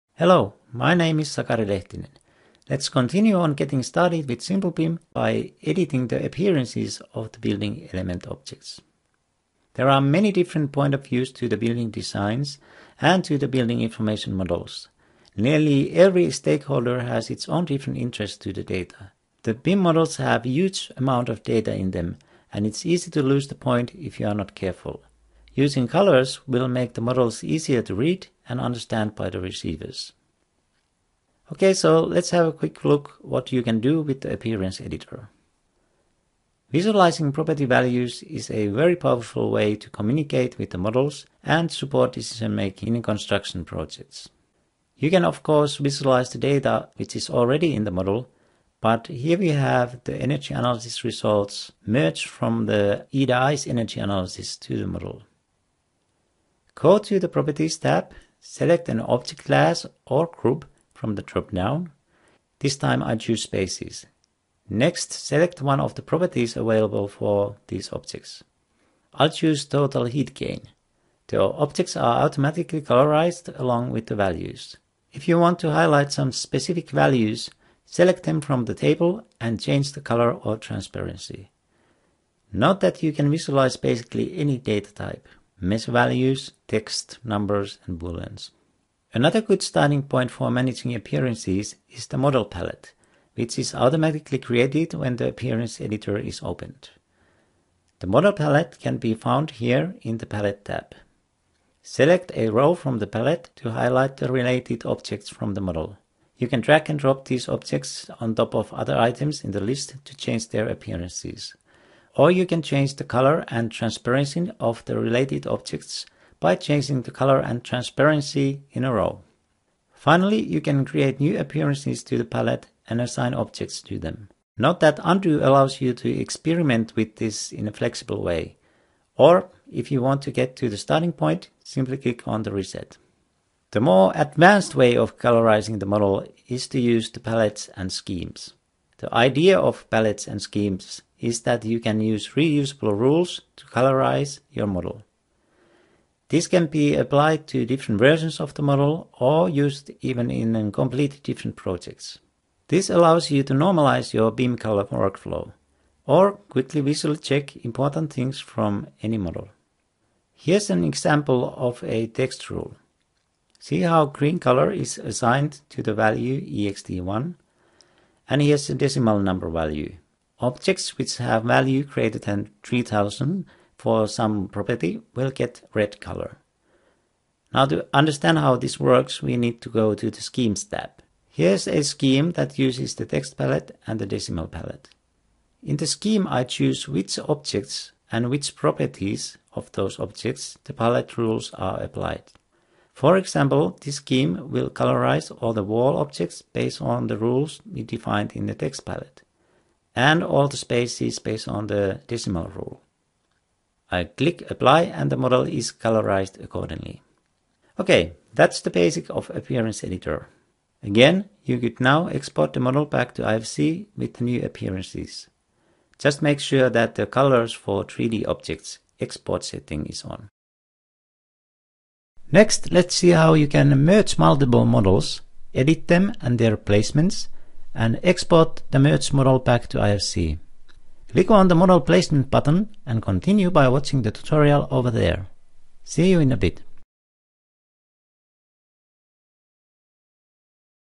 Getting Started with Simplebim 6 (8/11) - Appearance Editor
8/11 of the getting started with simplebim 6 tutorials. You will learn how to edit color and transparency of the objects and how to easily visualize the model data.